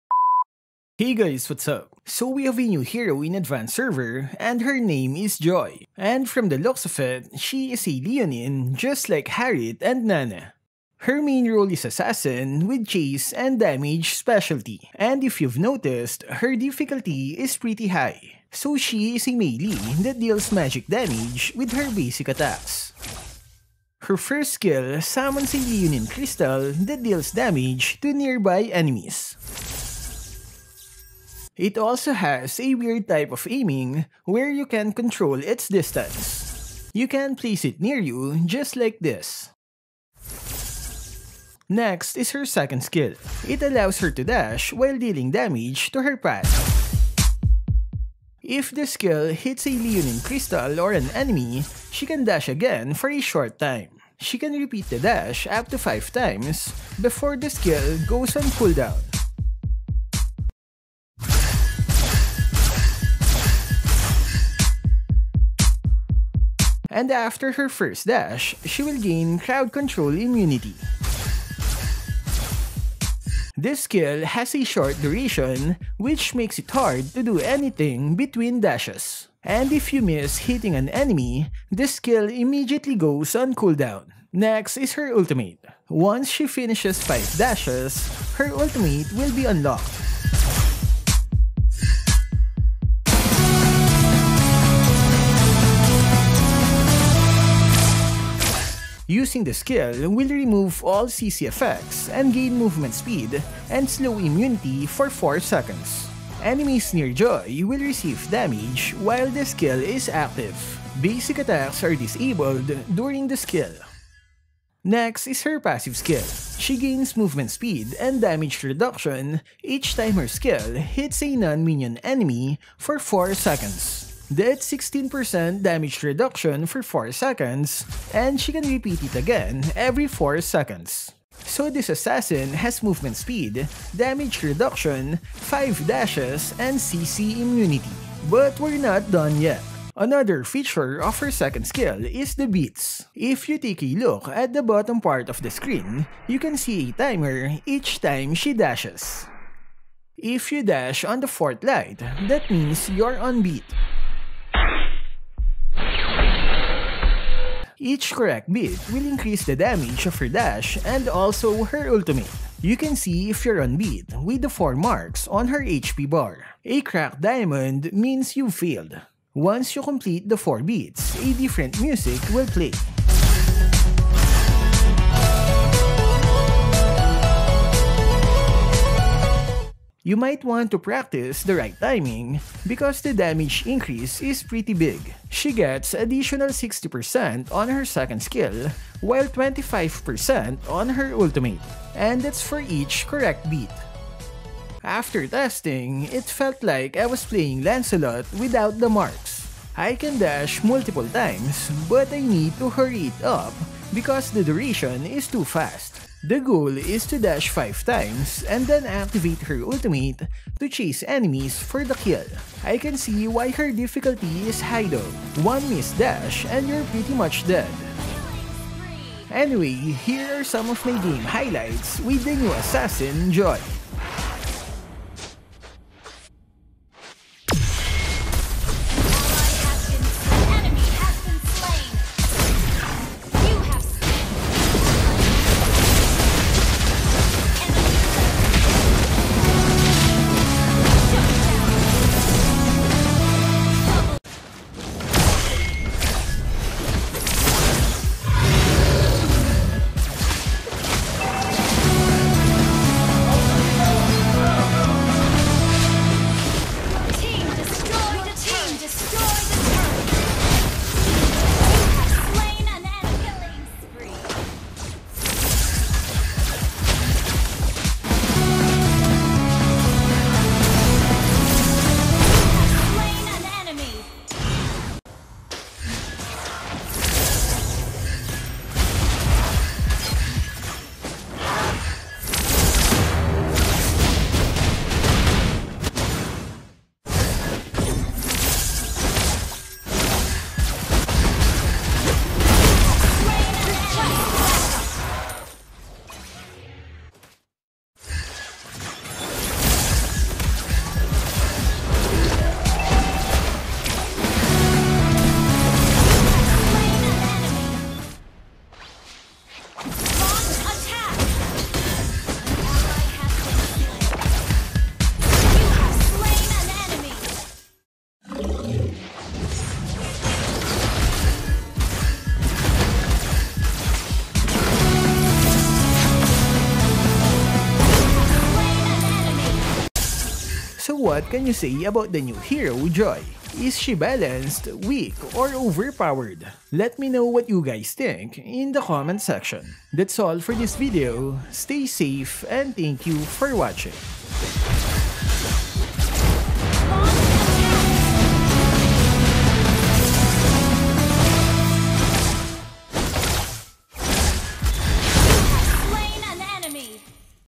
NEW HERO JOY HAS FIVE DASHES, CC IMMUNITY AND DAMAGE REDUCTION
So we have a new hero in advance server and her name is Joy
And from the looks of it she is a Leonin just like Harith and Nana